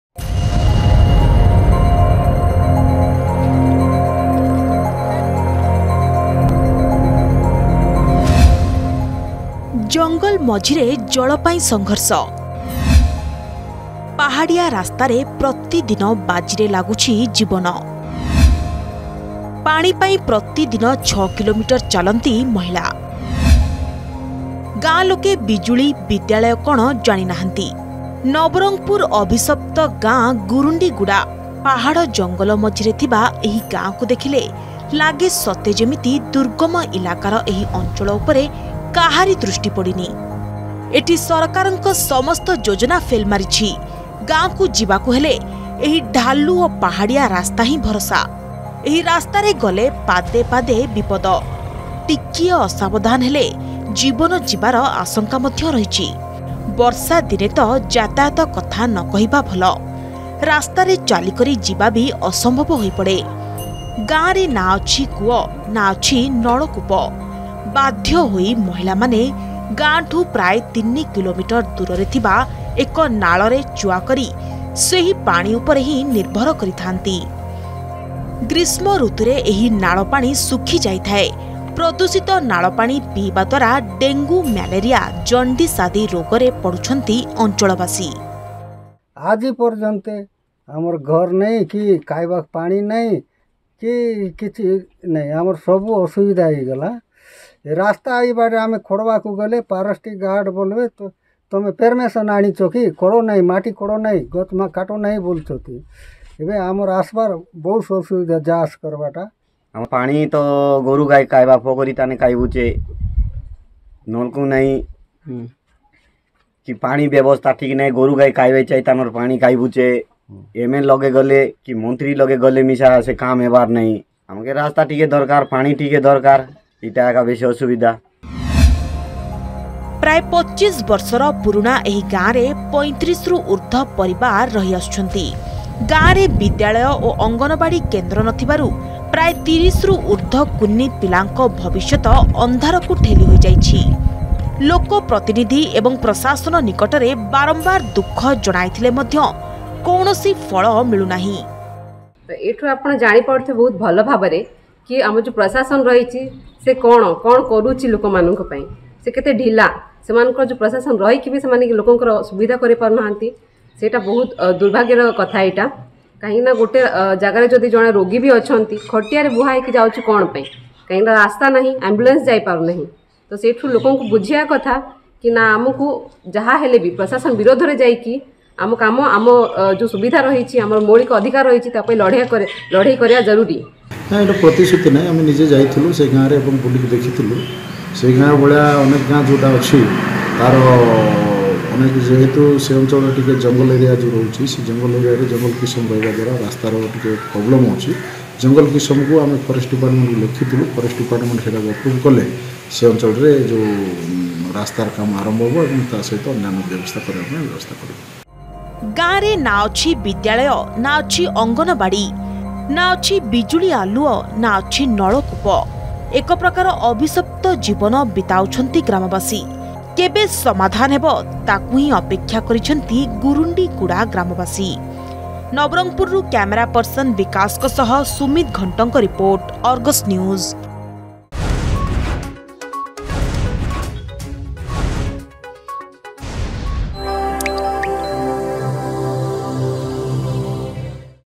ନବରଙ୍ଗପୁରର ଅଭିସପ୍ତ ଗାଁ ଗୁରୁଣ୍ଡିଗୁଡା।
ଜଙ୍ଗଲ ମଝିରେ ଜଳ ପାଇଁ ସଂଘର୍ଷ
ପାହାଡିଆ ରାସ୍ତାରେ ପ୍ରତିଦିନ ବାଜିରେ ଲାଗୁଛି ଜୀବନ
ପାଣି ପାଇଁ ପ୍ରତିଦିନ ୬ କିଲୋମିଟ ଚାଲନ୍ତି ମହିଳା
ପାହାଡ ଜଙ୍ଗଲ ମଝିରେ ଥିବା ଏହି ଗାଁକୁ ଦେଖିଲେ ଲାଗେ ସତେ ଯେମିତି ଦୁର୍ଗମ ଇଲାକାର ଏହି ଅଞ୍ଚଳ ଉପରେ କାହାର ଦୃଷ୍ଟି ପଡିନି। ଏଠି  ସରକାରଙ୍କ ସମସ୍ତ ଯୋଜନା ଫେଲ ମାରିଛି । ଗାଁକୁ ଯିବାକୁ ହେଲେ ଏହି ଢାଲୁ ଓ ପାହାଡିଆ ରାସ୍ତା ହିଁ ଭରସା